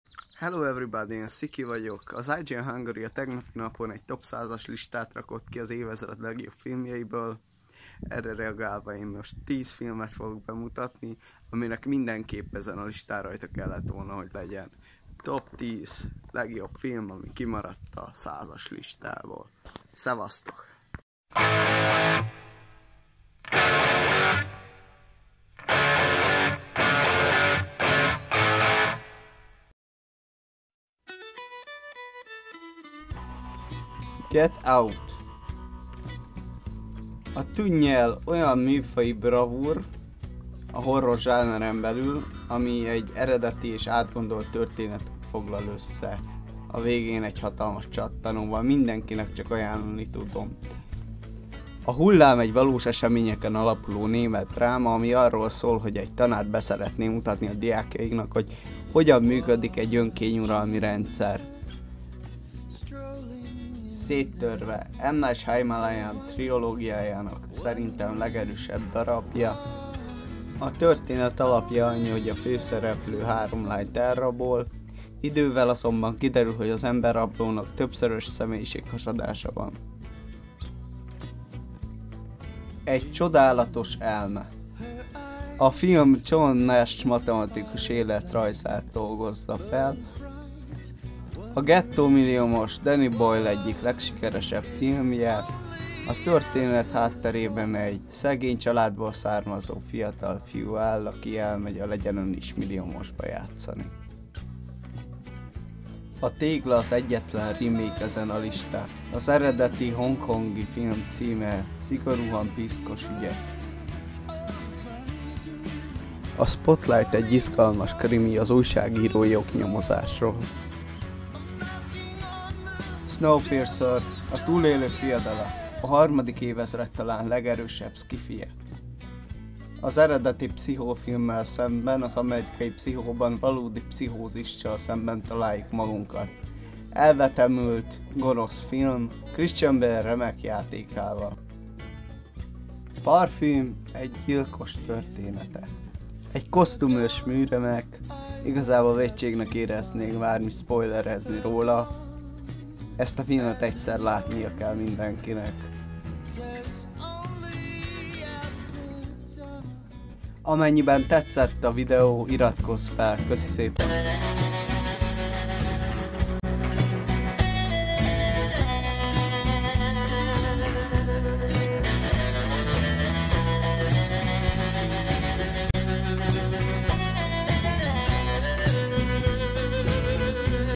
TOP 10 film ami kimaradt egy bizonyos 100as listáról
Az IGN Hungary által ismertetett top 100as listán nem szereplő legjobb filmek, amik a 3. évezredben készűltek.